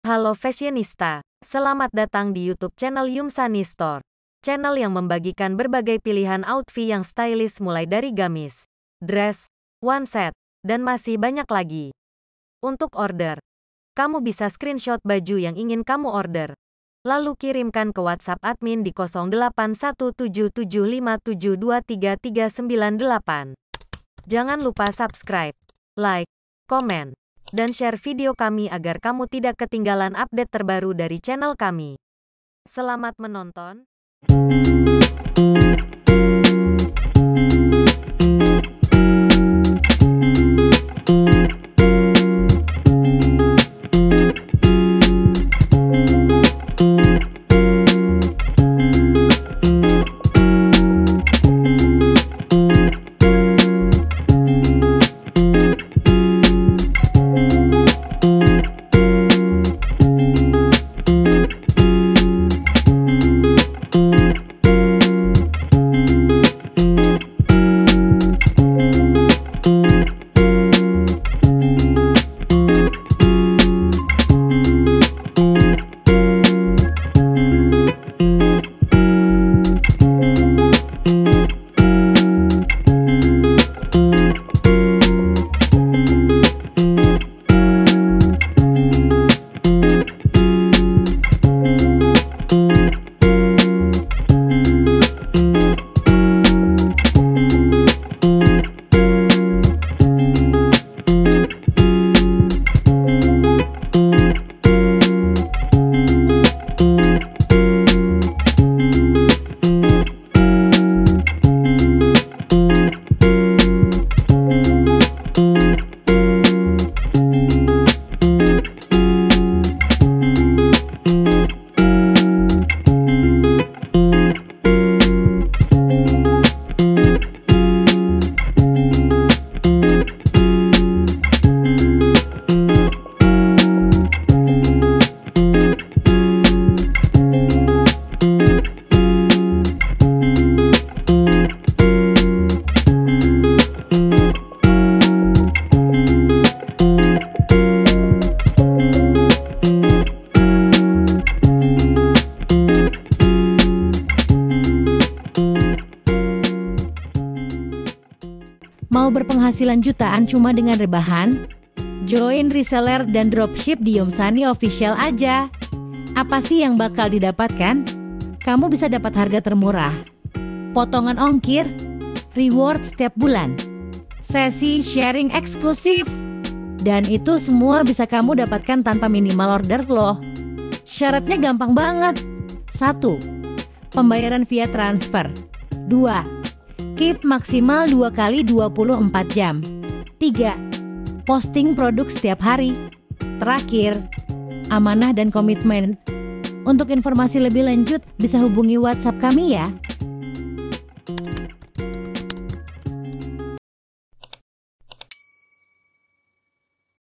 REFERENSI MODEL GAMIS SYARI MODERN TERBARU DAN KEKINIAN 2024 // Part 10
Halo fashionista! 🛍️

Kalian pasti suka dengan pilihan outfit yang stylish tapi tetap budget-friendly, bukan? Nah, di video kali ini, Yumsani Store akan membagikan rekomendasi model gamis super cantik dengan harga hanya terjangkau! 😍

Lazada: Yumsani.store
Shopee: Yumsani.store
TikTokShop: Yumsani.store02

Yumsani Store